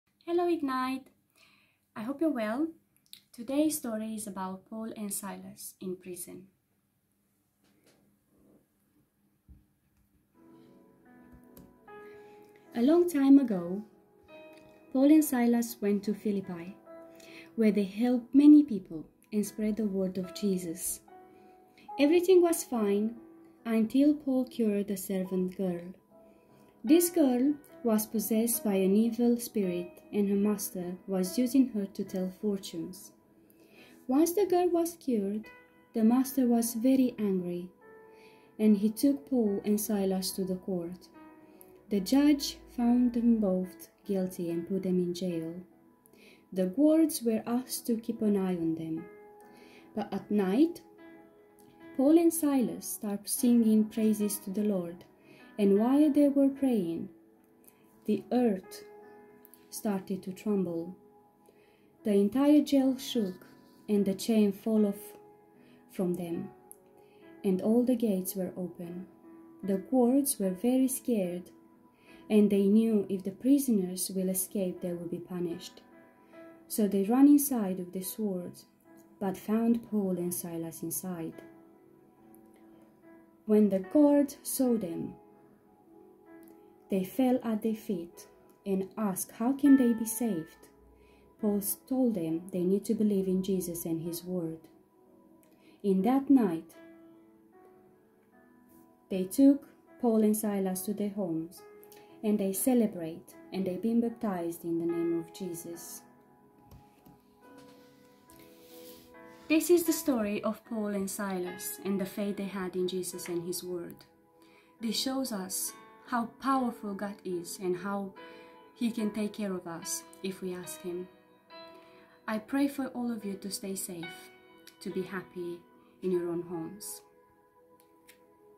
31st January 2021, Ignite
Andreea talks to us about Paul and Silas in prison.